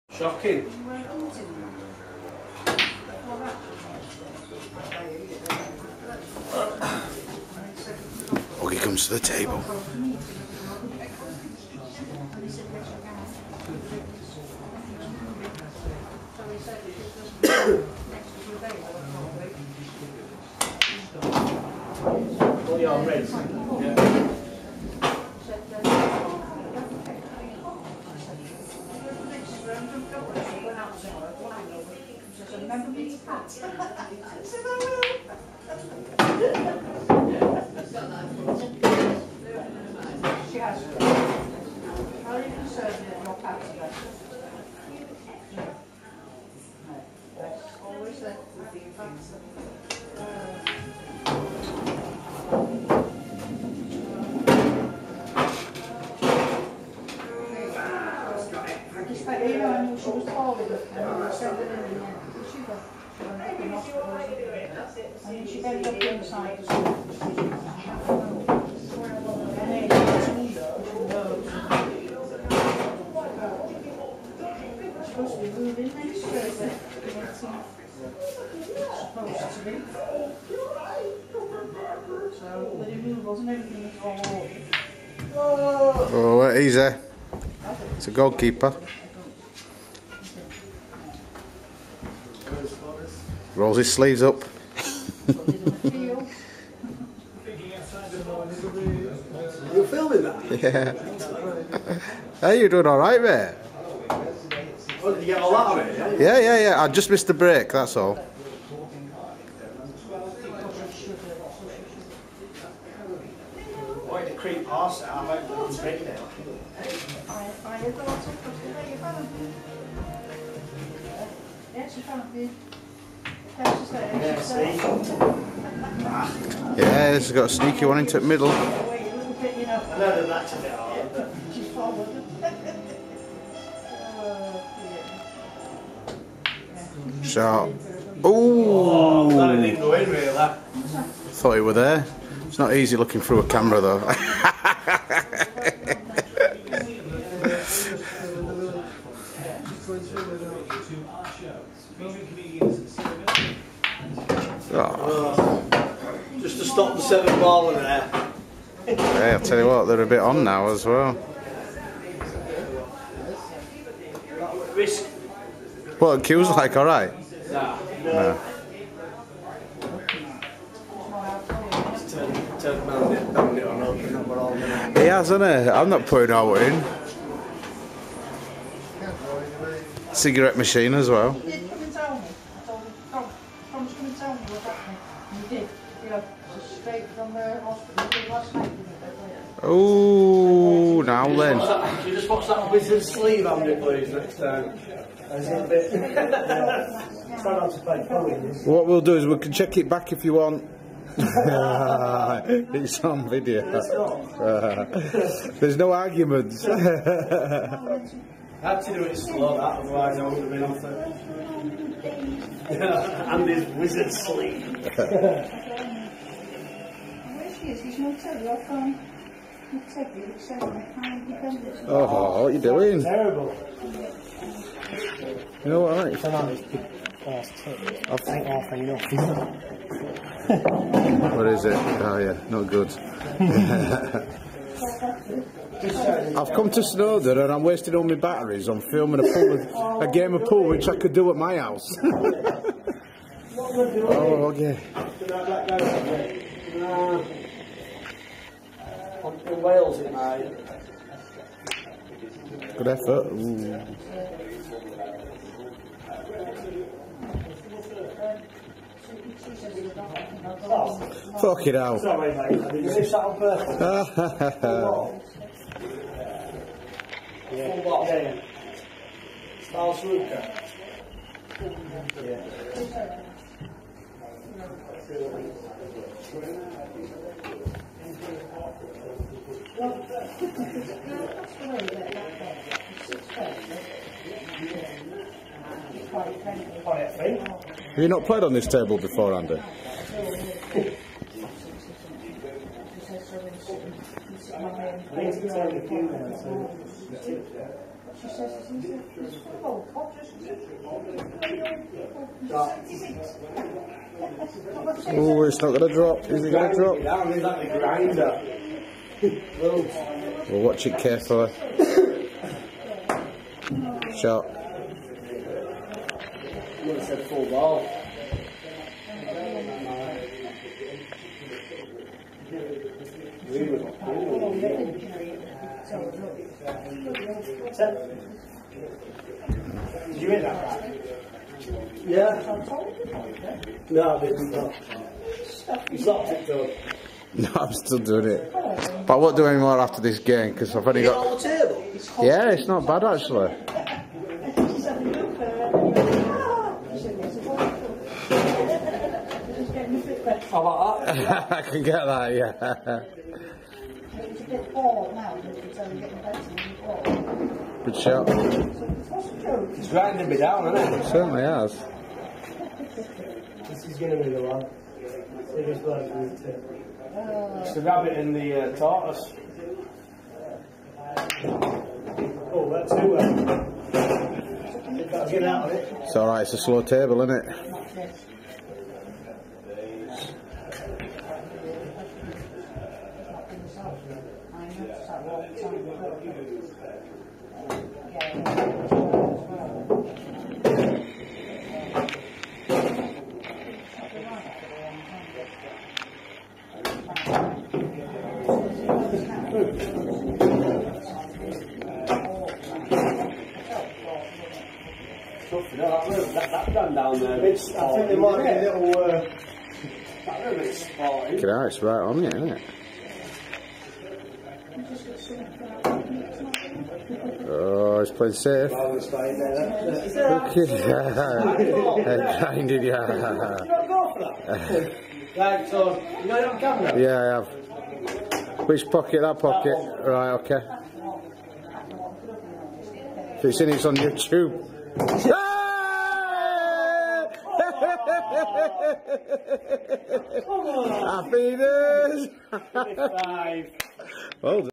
Oggy Llanberris Pool
trick shot black ball at end. winner !!!